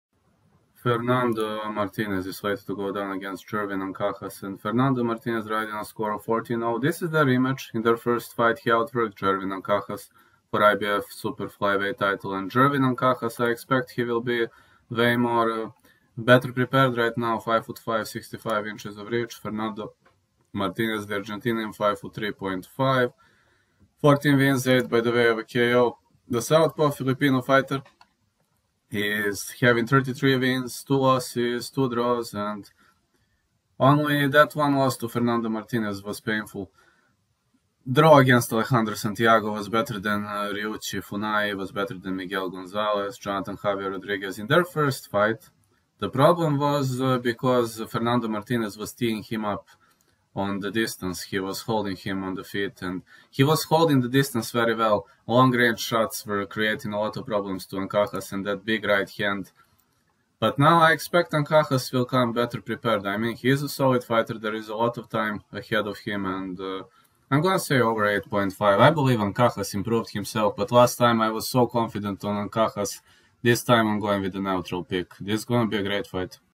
Fernando Martinez Vs Jerwin Ancajas 2 - Prediction
The Bulgarian Cowboy says over 8.5 rounds.